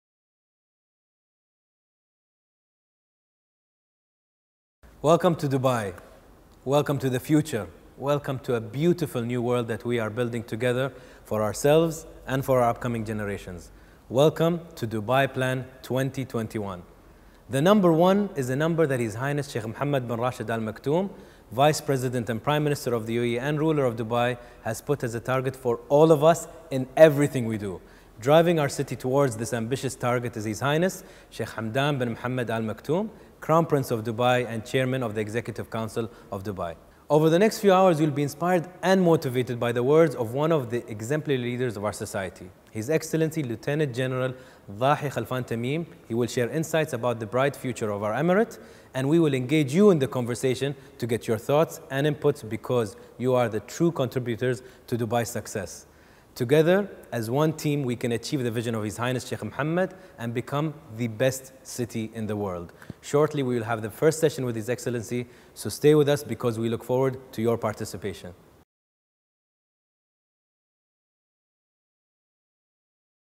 Dubai Plan 2021 - Anas Bukhash presents some insights
Media personality Anas Bukhash presents some insights from His Excellency Dhahi Khalfan Tamim in our session 'Dubai: Moving Forward', an interactive online brainstorm about Dubai Plan 2021.